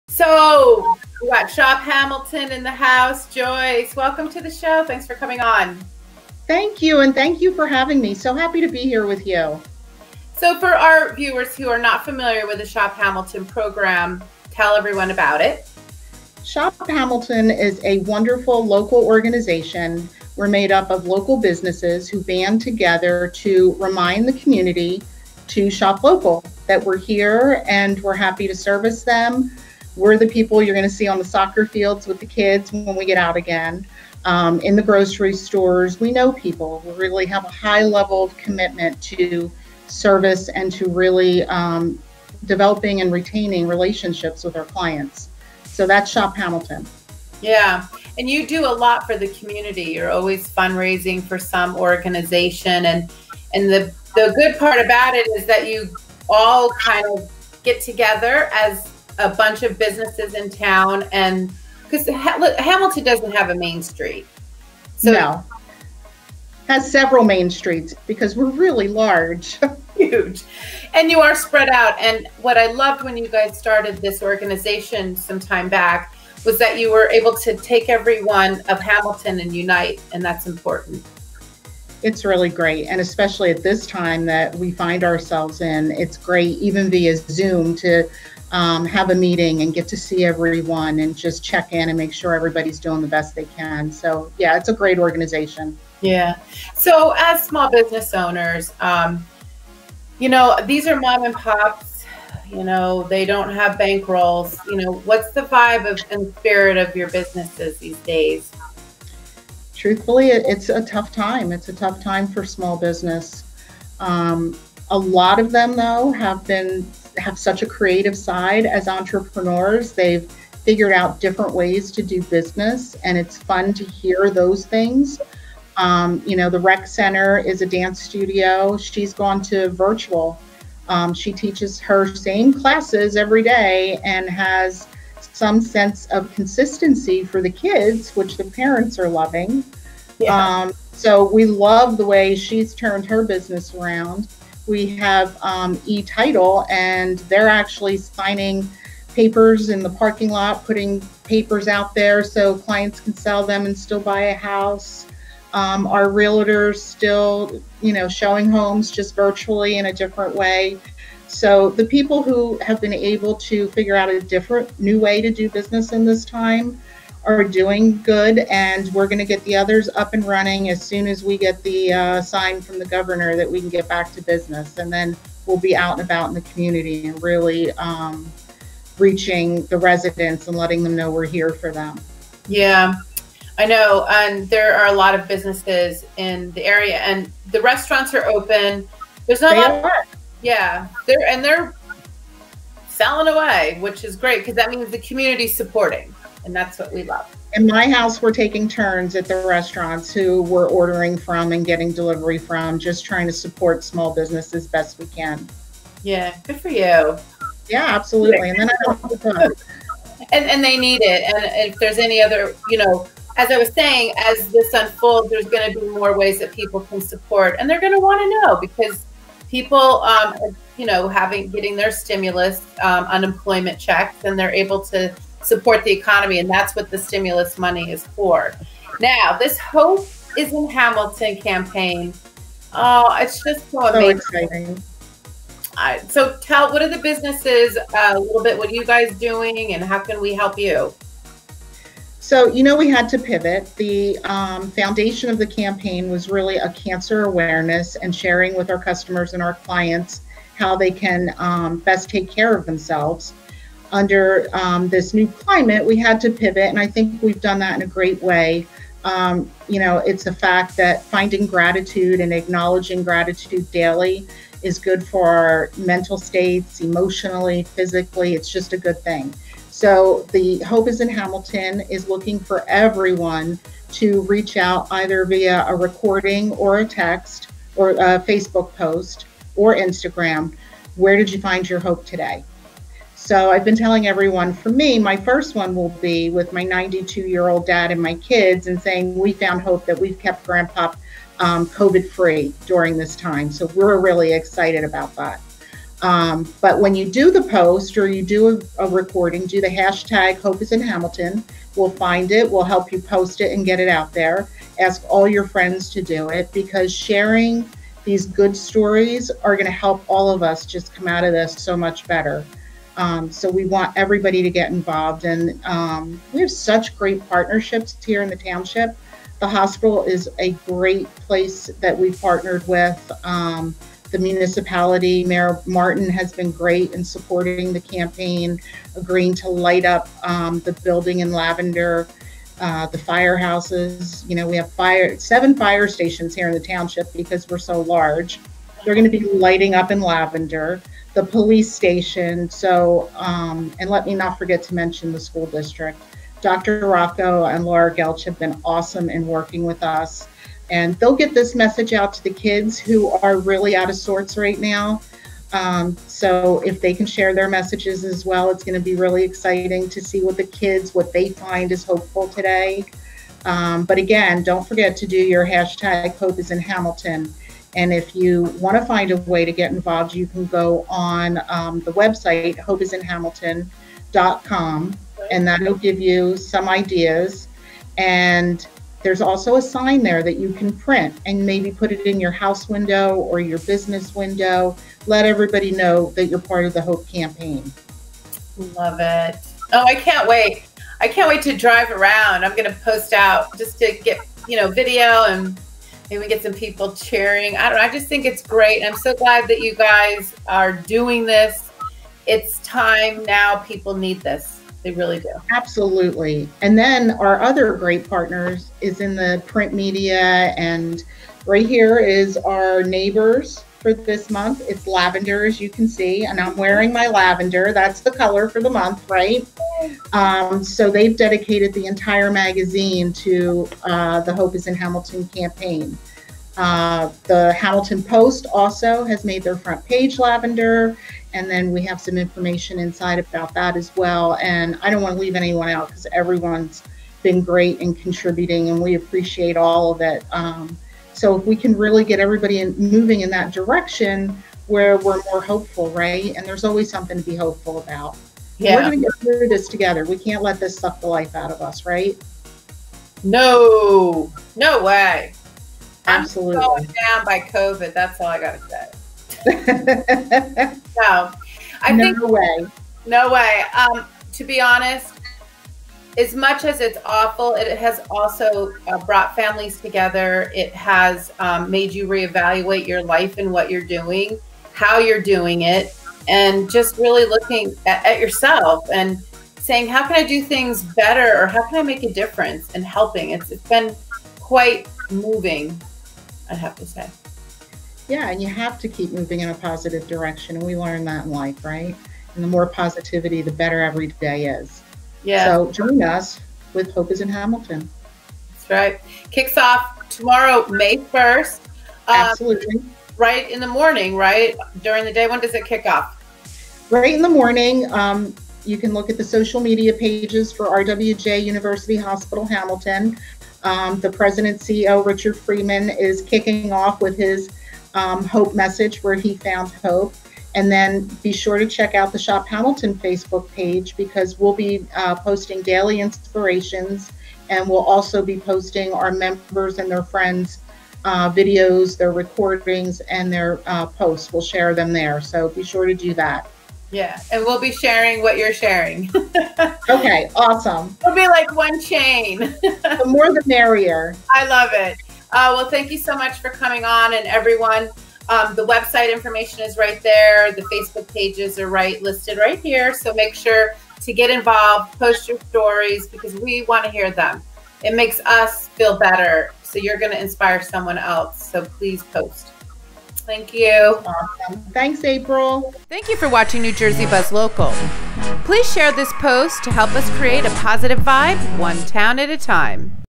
Shop Hamilton Small Businesses Hope is In Hamilton
Joyce Cantalice, President of Shop Hamilton unites with RWJ of Hamilton and Hamilton Township to tell the stories of the Hope that Is In Hamilton. These are the stories of the Hope that reside within Hamilton Township, New Jersey!

We are always looking for Positive stories to cover!  Events, life, people and destinations.